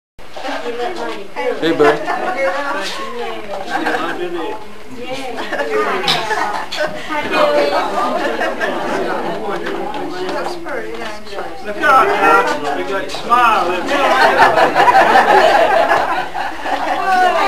1989 Drew Daley MargaretBuren 50th Anniversary
1989: Drew Daley attending Margaret and Buren Lee's 50th Anniversary Party at North Newington Baptist Church in Newington, GA.  November 18, 1989.  Also in video: Lina & Walter Lee